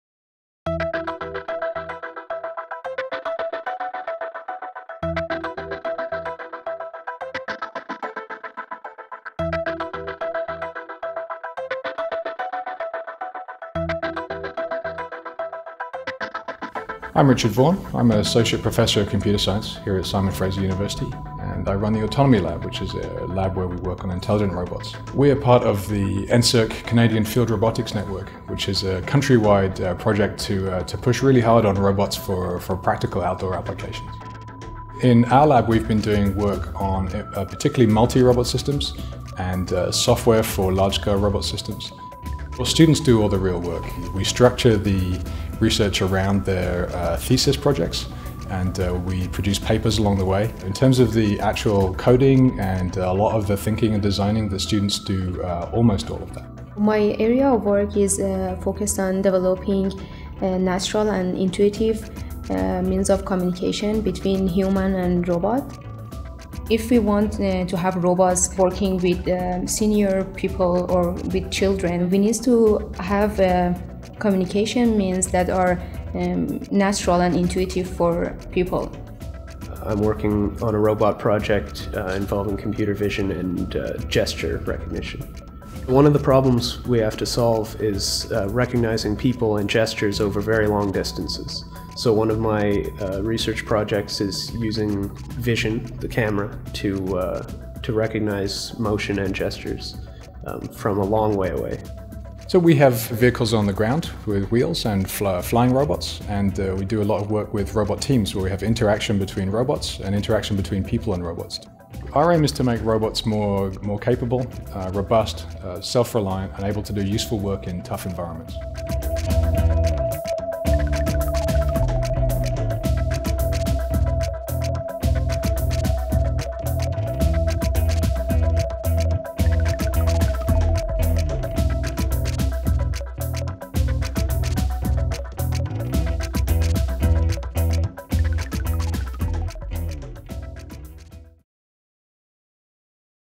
FAS Robotics Autonomy Lab
Interested in robotics? This video explores the autonomy lab at SFU's Faculty of Applied Sciences, which aims to develop capable, robust and self-reliant robots.